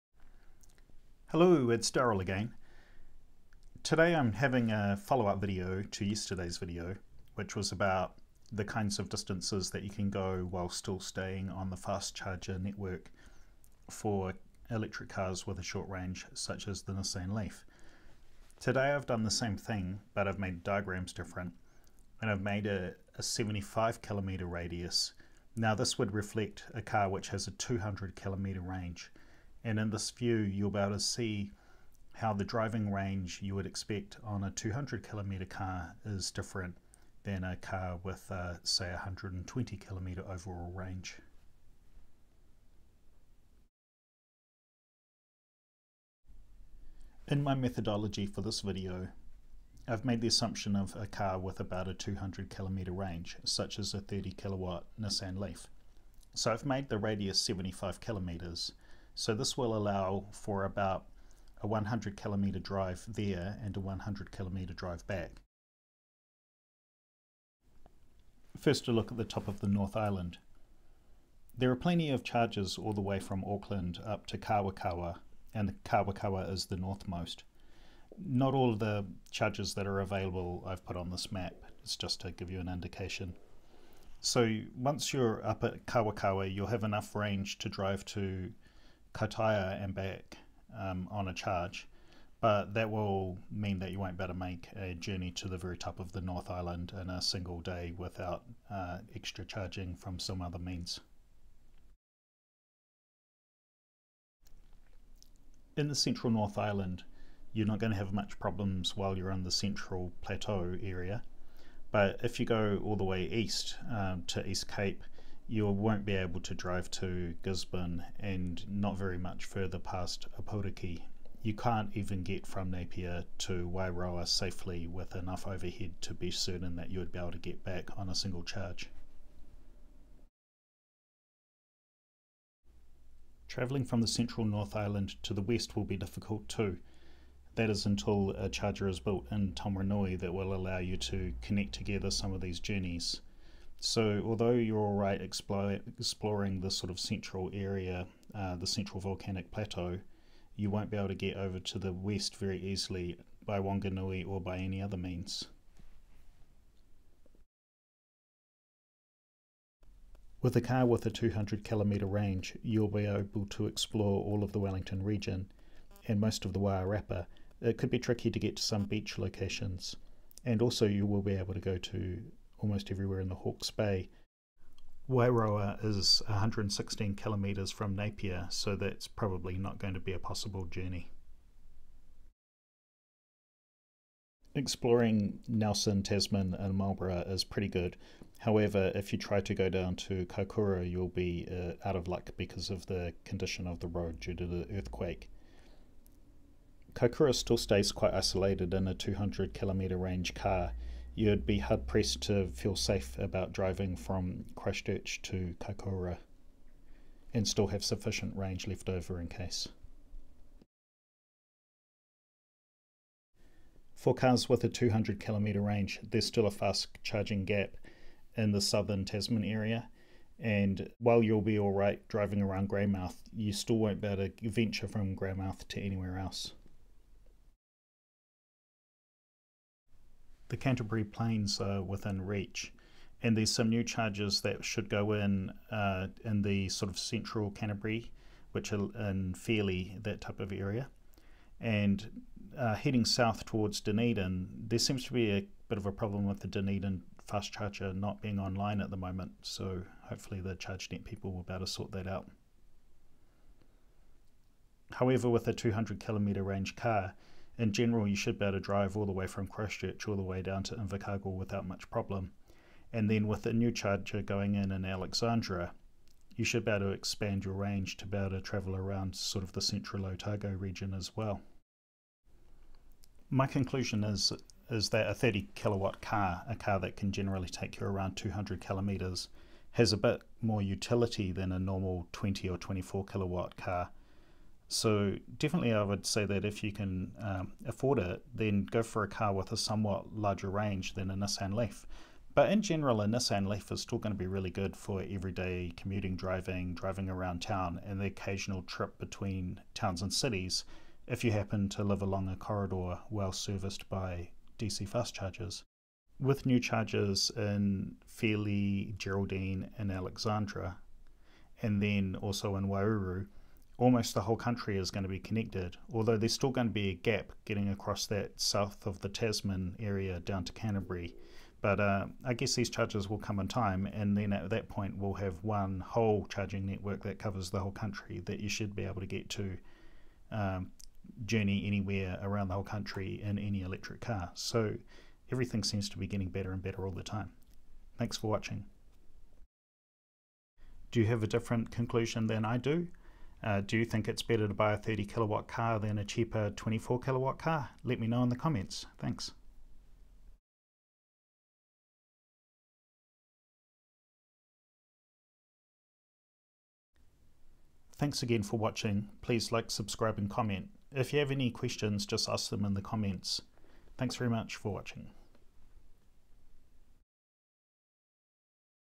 Is 200km Enough EV Range?
Investigation of distance between DC fast chargers, with a 30kWh battery, 200km range Electric car:
Nissan Leaf 30kW version.
Hyundai Ionic.
BMW i3 94Ah